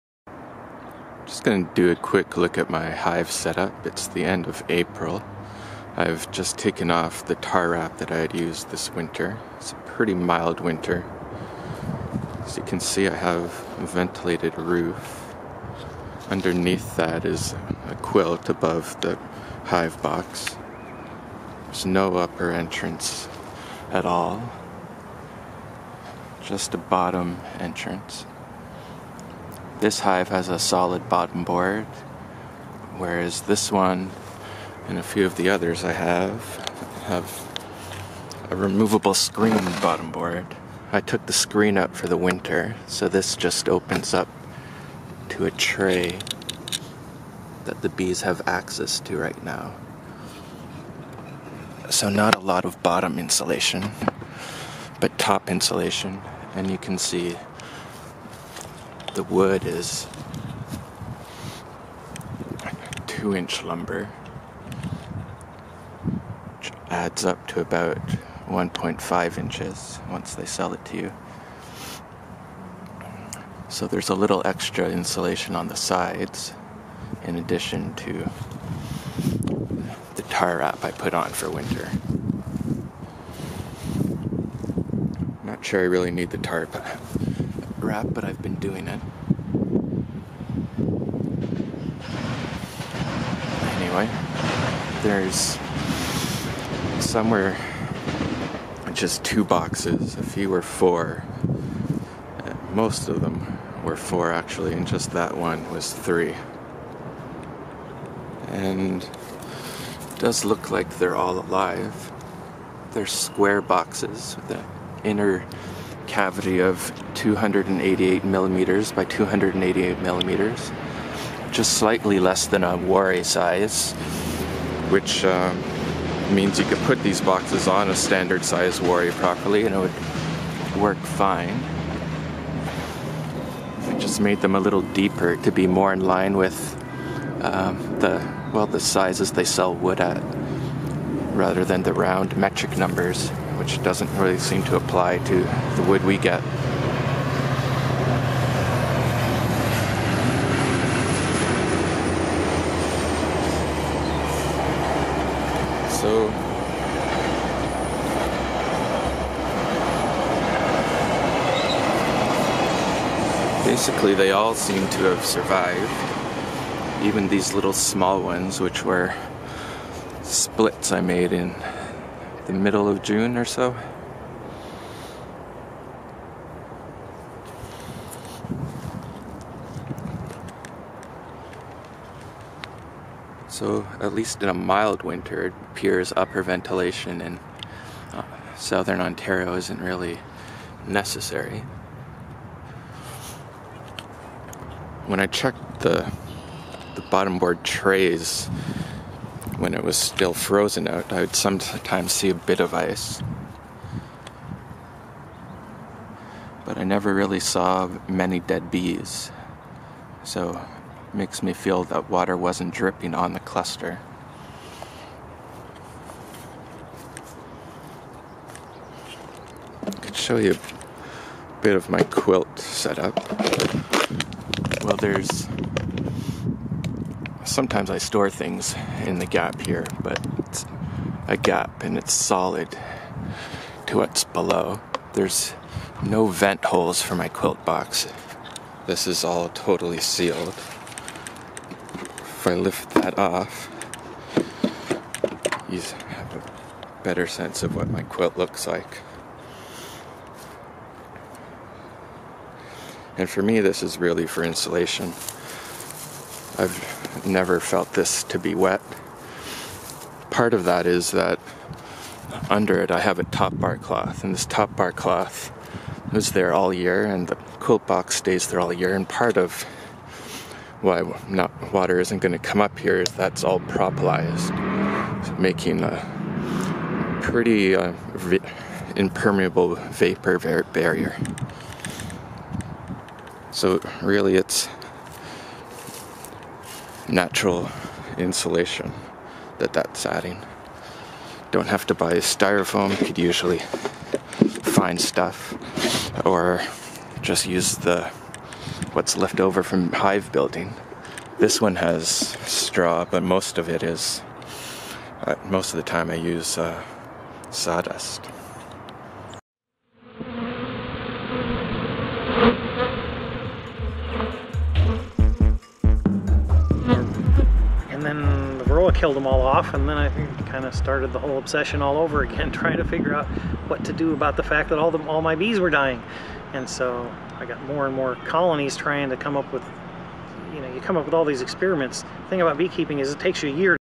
My hive setup | In the Apiary
Checking to see how the hives are doing as it warms up this spring and showing off my version of the warré hive.

The graph by David Heaf that was mentioned in the video